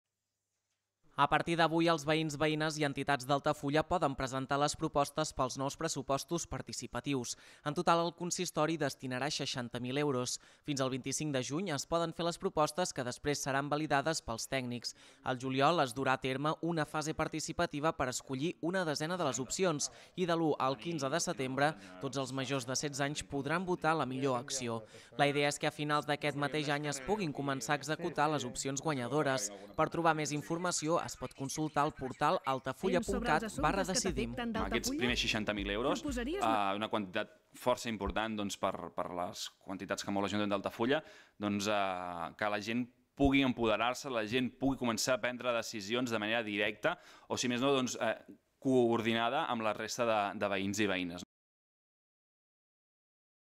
La ciutadania d'Altafulla ja pot decidir com invertir 60.000 euros
A partir d’avui i fins al 25 de juny els veïns, veïnes i entitats d’Altafulla poden presentar les propostes per als nous pressupostos participatius. El consistori hi destinarà 60.000 euros i després seran validades pels tècnics.

Durant el mes juliol es completarà la fase participativa per escollir una desena de les opcions; de l’1 al 15 de setembre, tots els majors de 16 anys podran votar la millor acció.
La idea és que a finals d’aquest mateix any es puguin començar a executar les opcions guanyadores. Per trobar més informació es pot consultar el portal